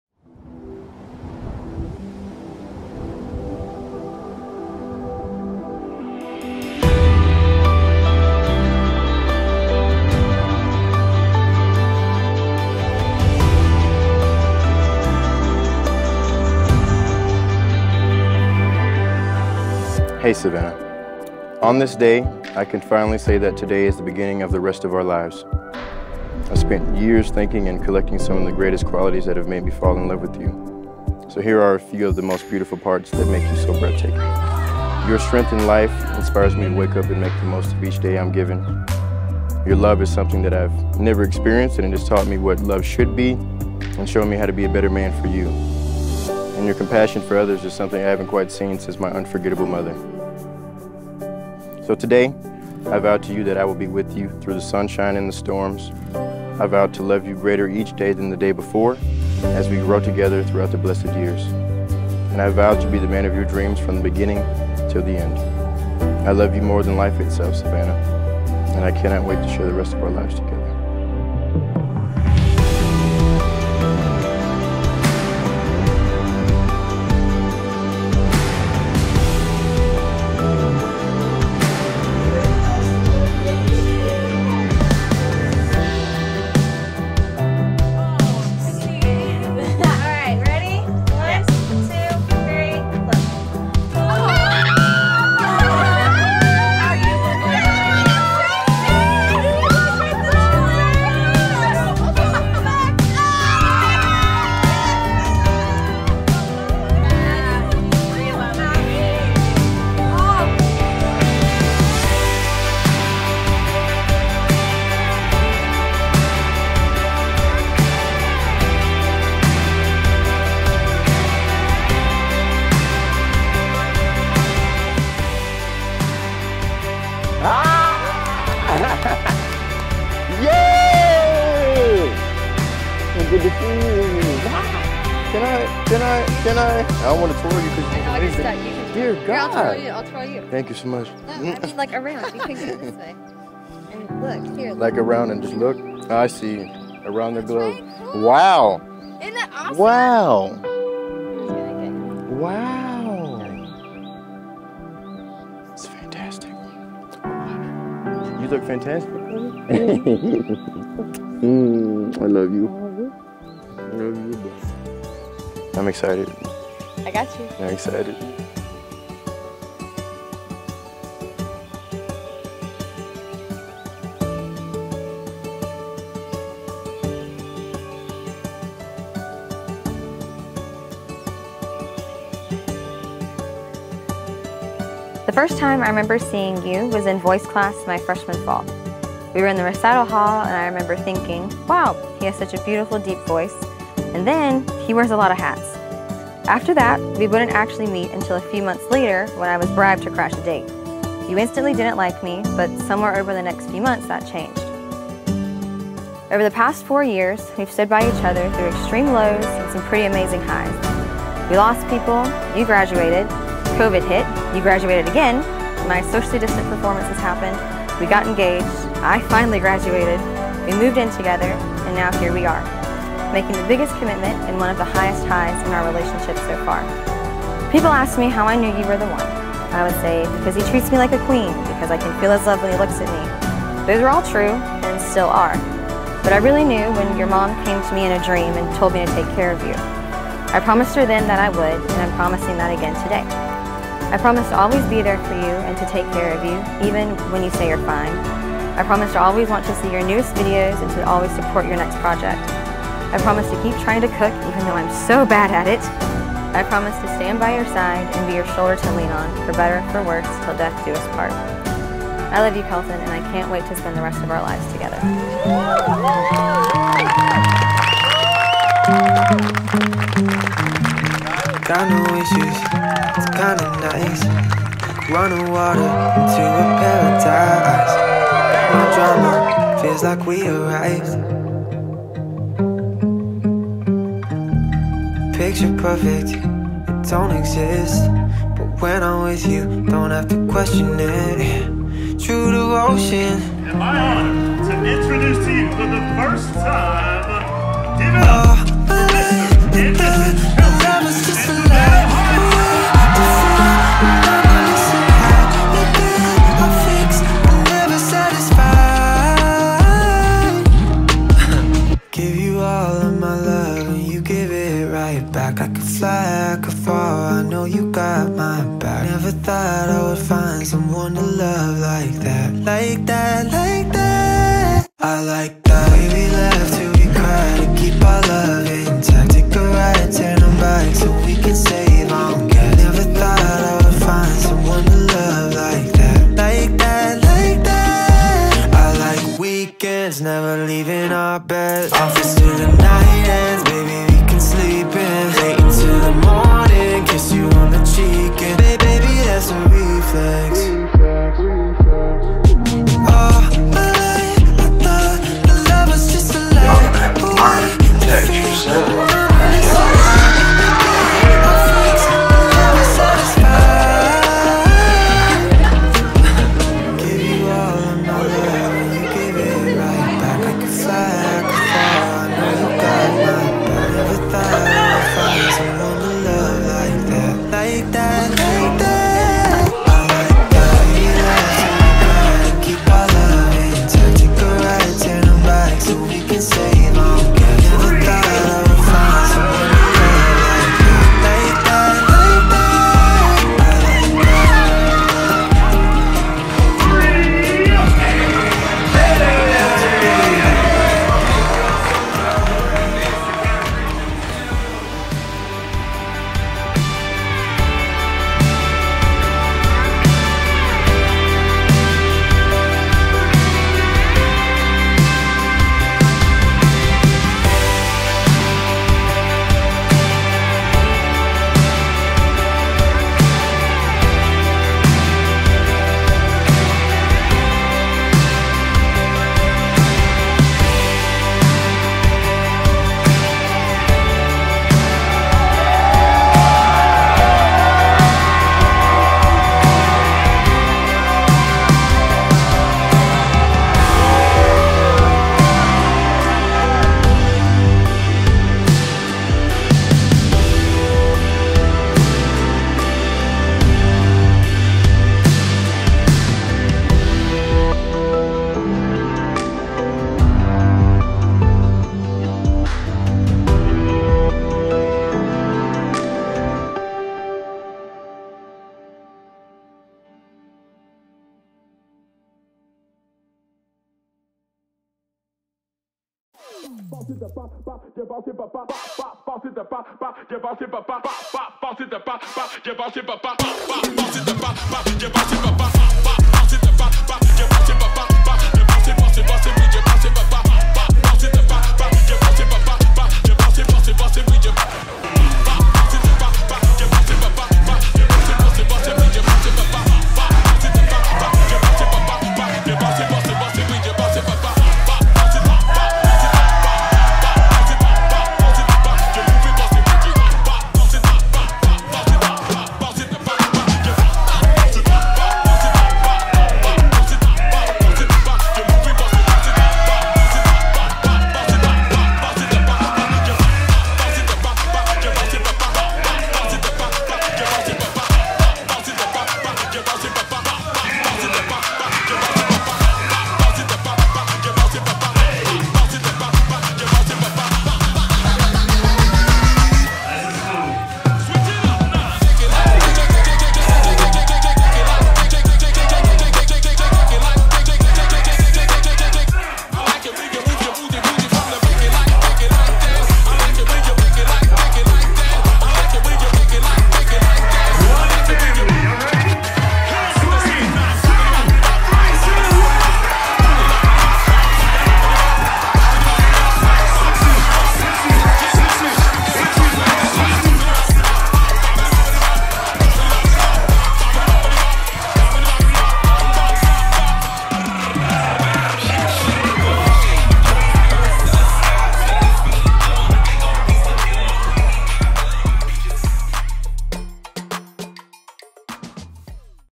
Savanna + Kelton Wedding Film
This is the official wedding video of Savanna and Kelton Hunt created by Joseph Choi.